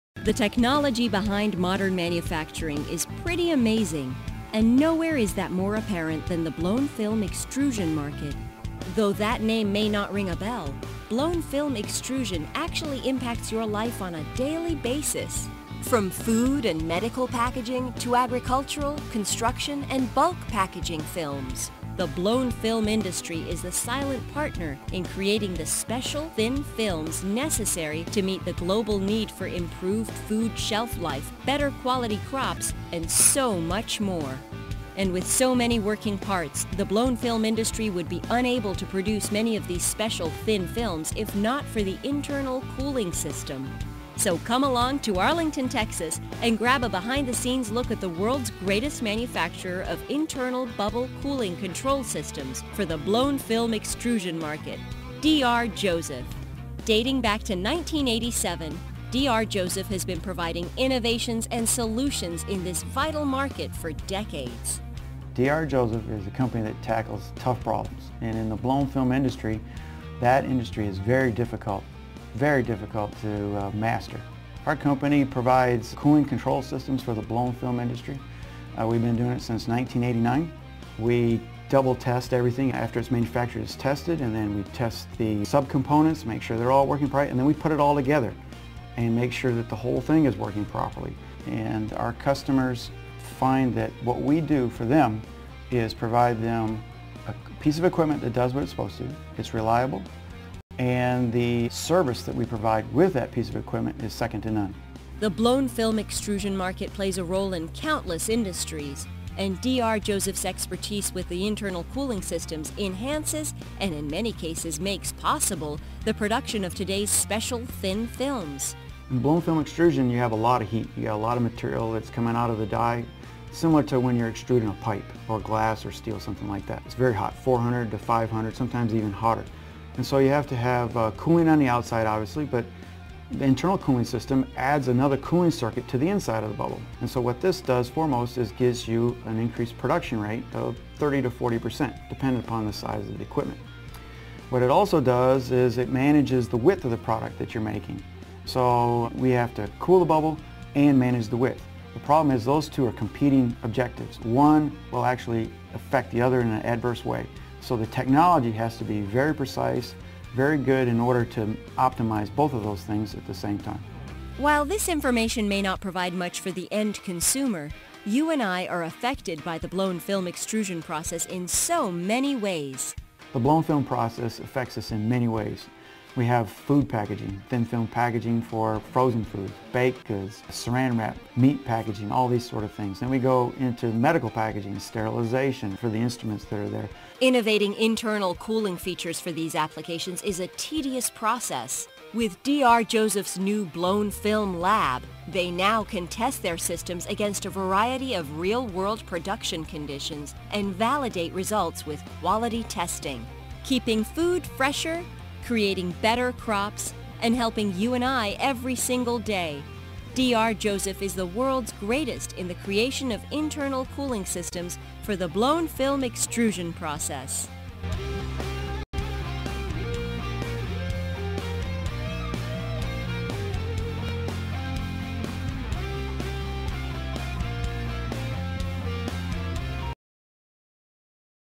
Worlds Greatest TV Show: D R Joseph, Inc.
How thin plastic films are made, applications and how internal bubble cooling systems from D R Joseph play a significant role in creating critical products for life.  Quick view of the new DR Joseph Blown Film Labs.